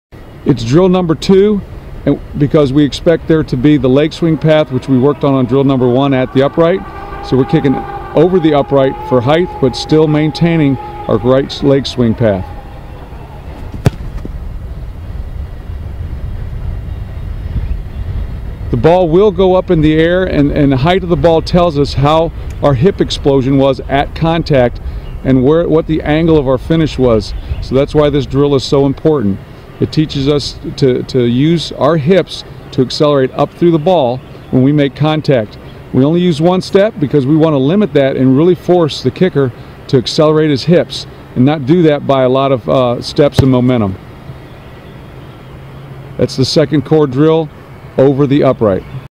How to Kick A Football: Over the Upright Football Kicking Drill 2 with Coach Bill Renner
This is the second kicking drill to do with the ball.  It develops the position of the accelerator (non-kicking) foot, a consistent rotation of the hips, energy transfer from the accelerator foot, to the hips, to foot-ball contact and a smooth, rhythmical, consistent leg swing tempo.  Put the ball on the end line and sideline directly across from the goal post. Do this drill 1 set of 5 kicks before a field goal workout.  In training to improve do 1 set of 10 kicks.